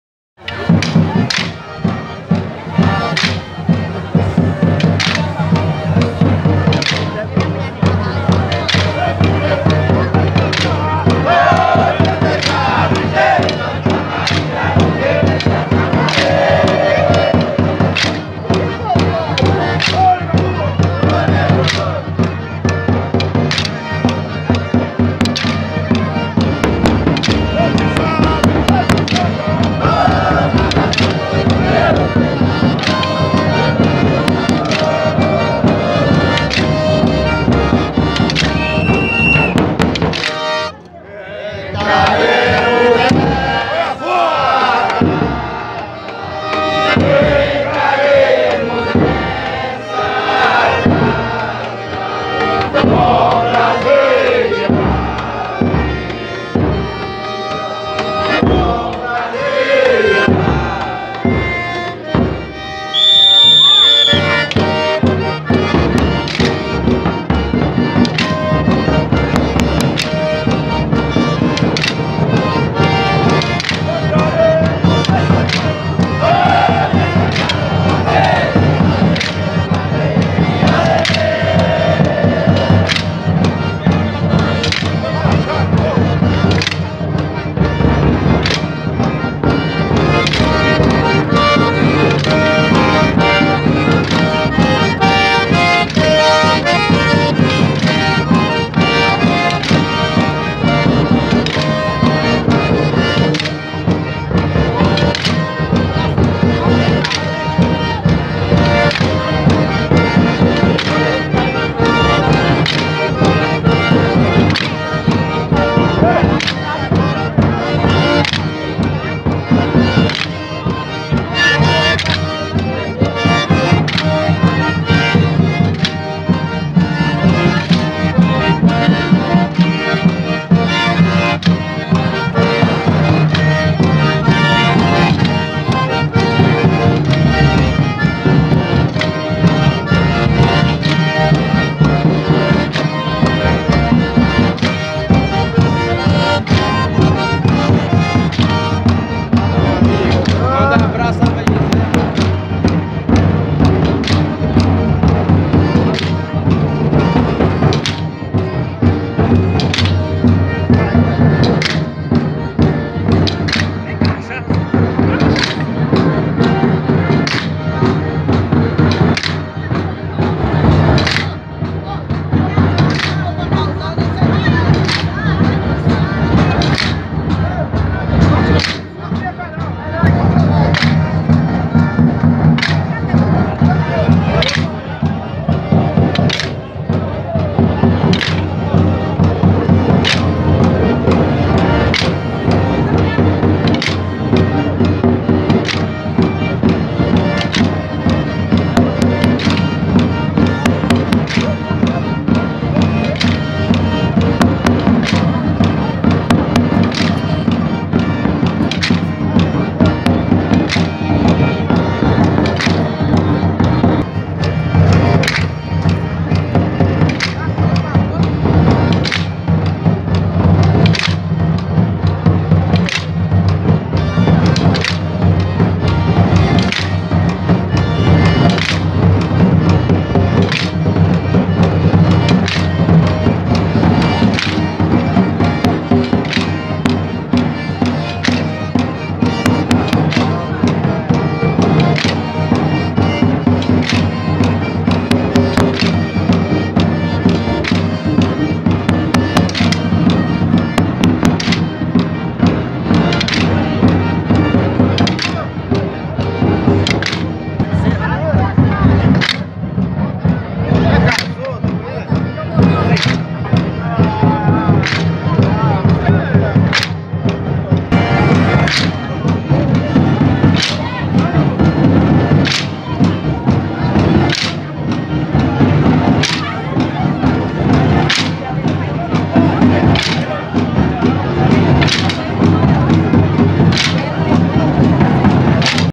FESTA DO ROSÁRIO DOS HOMENS PRETOS DO SERRO - CABOCLOS - 2022
Festa do Rosário dos Homens Pretos do Serro
CABOCLOS - representam os índios que ao som de caixas de couro e sanfona reverenciam a Virgem com alegria. São a persistência do indígena que não se deixou dominar. Enfeitam-se com coletes coloris, adornados com lantejoulas; usam cocares de penas coloridas, enfeitadas com fitas, perneiras com penas, pulseiras e brincos. Trazem consigo uma flecha da madeira pra acompanha o ritmo da caixa